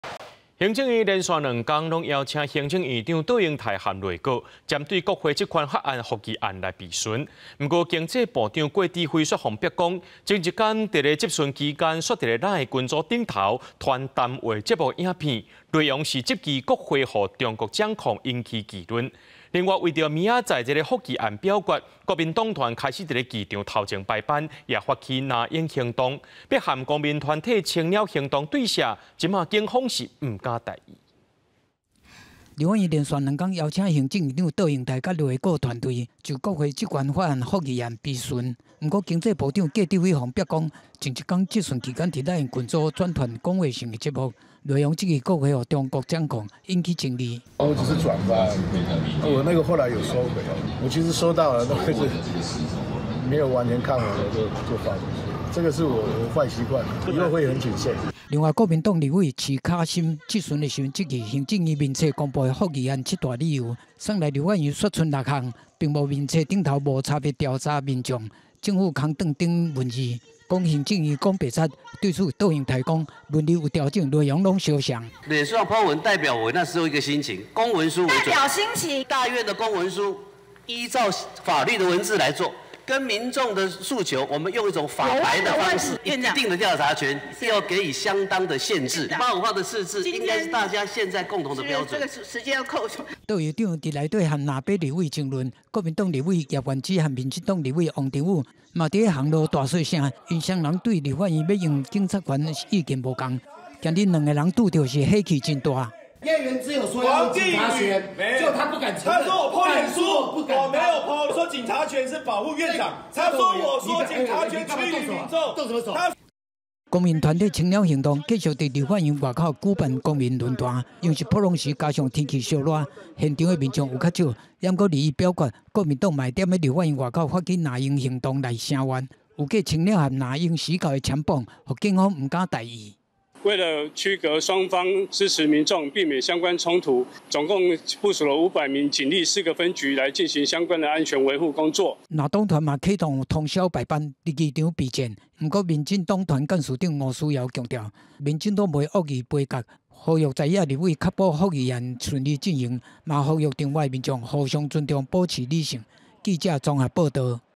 有人講國會予中國控制 郭智輝分享內容惹議 | 公視台語台 | 20240620
立法院連紲兩工攏邀請行政院長-卓榮泰和內閣，針對國會職權法案、覆議案來備詢，毋過經濟部長-郭智輝煞予人煏講，前一工佇質詢期間，煞咧Line的群組，轉談話節目的影片，內容是質疑國會予中國掌控，引起爭議。另外，為著明仔載的覆議案表決，國民黨團開始佇議場頭前排班，也發起「藍鷹行動」，欲和公民團體「青鳥行動」對削，警方是毋敢大意。

四節新聞 ：　逐工（每天）09:00 / 12:00 / 15:00（拜一至拜五） / 19:00